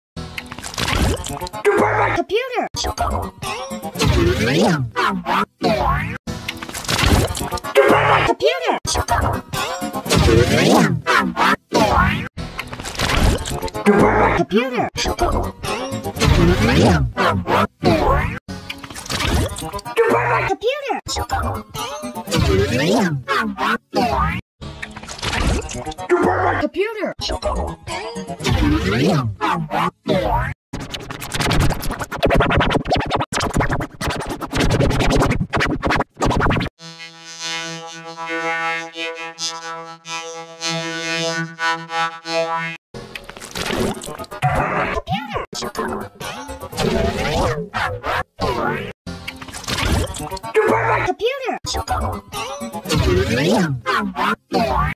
You Burnt My Computer! Csupo Effects
Credits to myself for the sources.
Gift for @TheMediaSamsungLogoEditor2024

Laptop users: Good morni- What Happened To My Laptop!? XD

List Of Effects:
1. Normal
2. G Major 4
3. Does Responded
4. Diamand Major
5. Not Scary (TV Simutaltior)
6. NSWIDTX
7. My Chorded
8. Robot Flip
9. LTKC Map

Follow Roblox / Miles10227